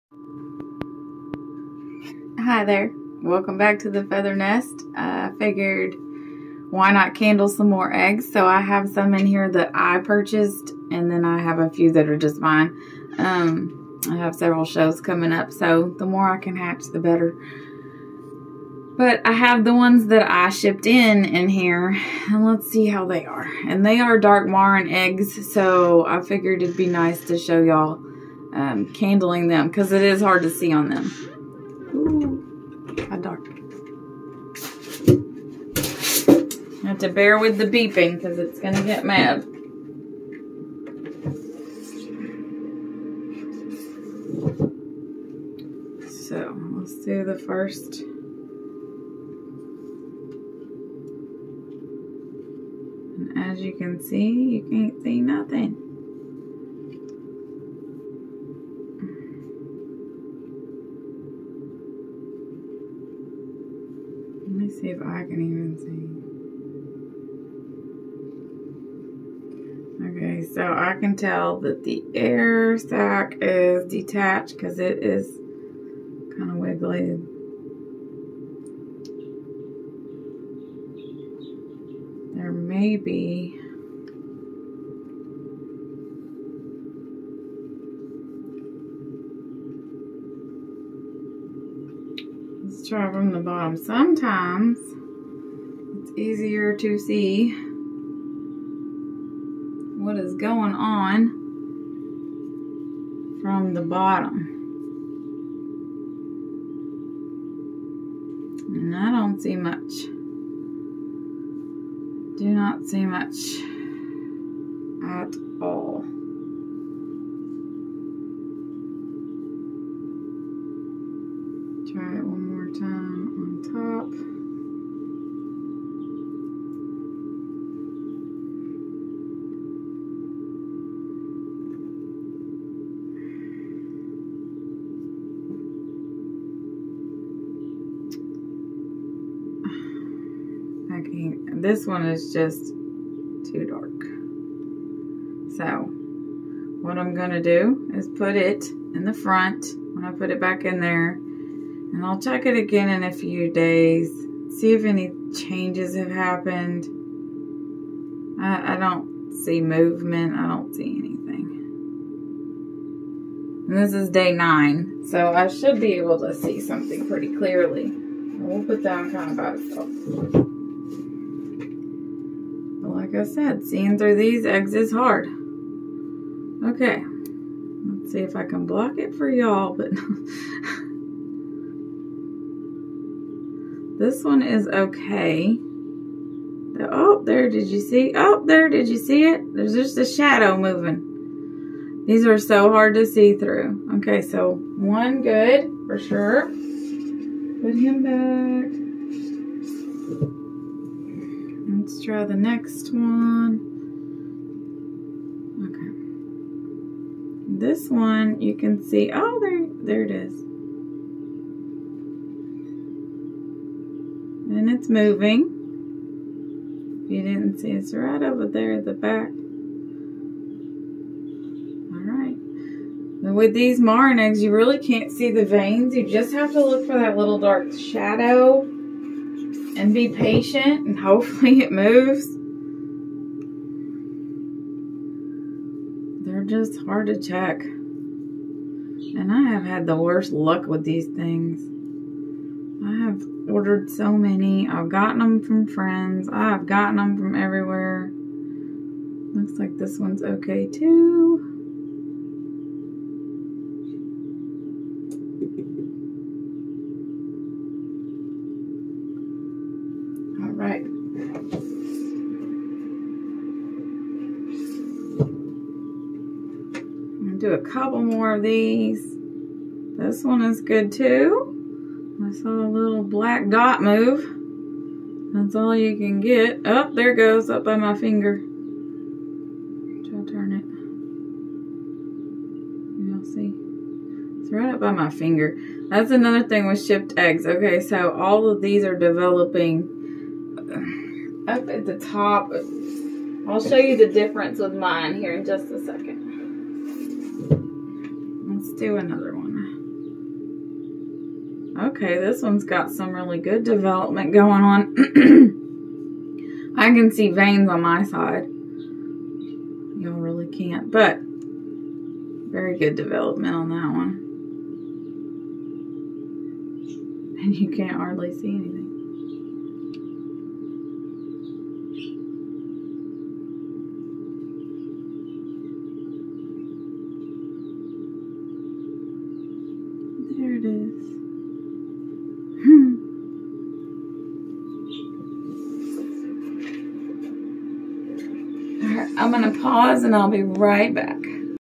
candling part 1
candling some dark Marans eggs